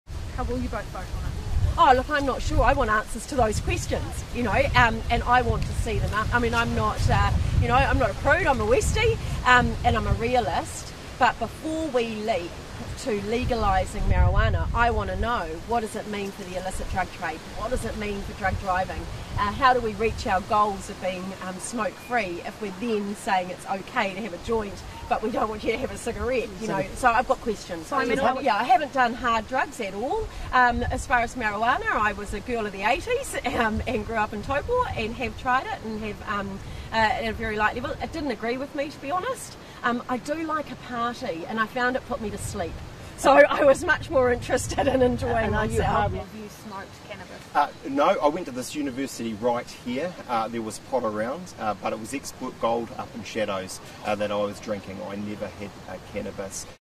Paula Bennett reveals her cannabis experience after being named National’s drug reform spokesperson
She was also asked how she would vote on the cannabis legalisation referendum, saying, “I’m not a prude, I’m a Westie and I’m a realist”.